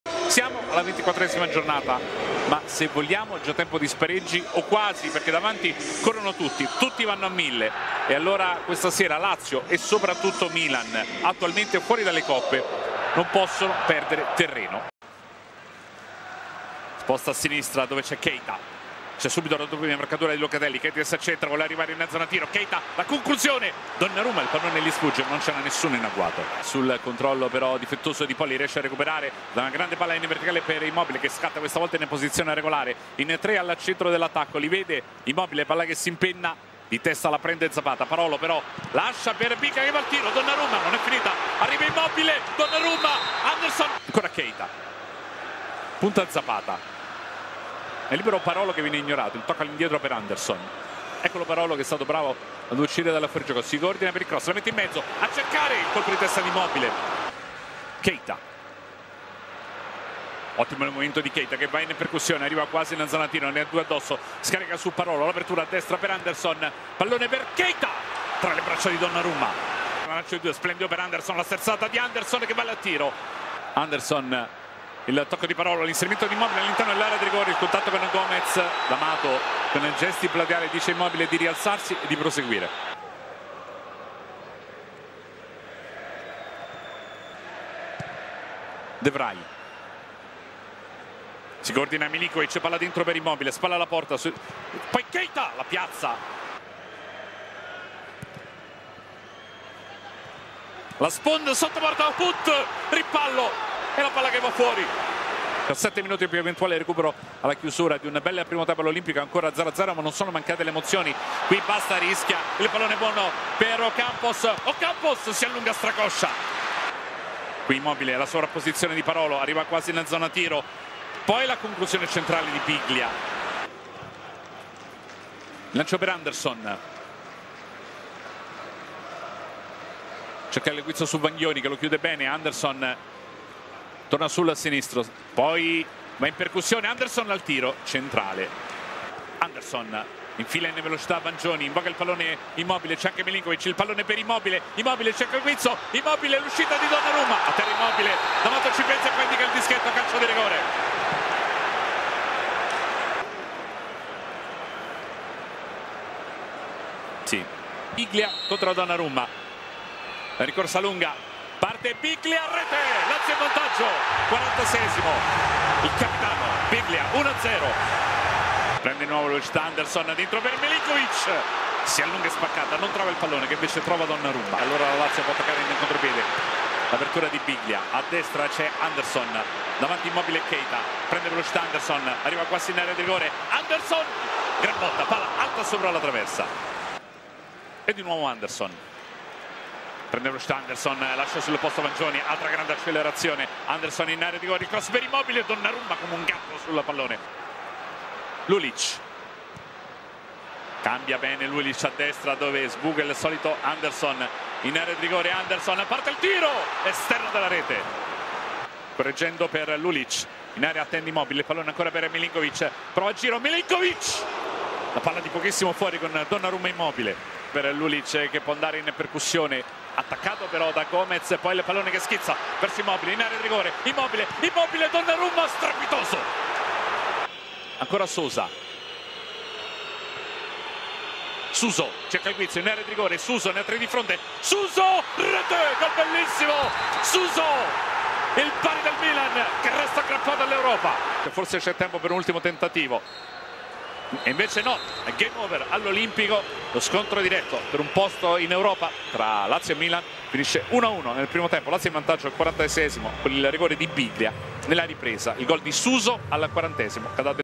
13/02/2017 - Lazio 1 Milan 1 - Serie A 2016/2017
Accadde oggi - 13 febbraio 2017
Mai pensare che il Milan sia finito. Come ha fatto il Bologna e come ha fatto la Lazio ieri sera, avversarie fin troppo compiacenti di una squadra che non finisce mai. Gol e vittoria al 90' in nove contro il Bologna; gol e pareggio all’Olimpico con una formazione senza sei titolari. Ieri il Milan ha rischiato tanto, l’ha sorretto quel gigante di Donnarumma, ma soprattutto l’ha salvato un’incredibile serie di errori degli attaccanti della Lazio, uno spreco mai visto, sia nel primo tempo quando la squadra di Inzaghi aveva il gioco in mano, che nel secondo quando aveva praterie per i suoi continui attacchi in contropiede. Il Milan di occasioni vere ne ha create pochissime e ha segnato con un capolavoro di Suso che dal niente ha inventato un gol straordinario. Sul piano del gioco, la partita è stata quasi tutta della Lazio, ma proprio questo ha appesantito le sue colpe. Ha buttato via una partita che non poteva e non doveva buttare via: Atalanta e Inter sono ancora avanti, il Milan non si è staccato, la Fiorentina di un solo punto.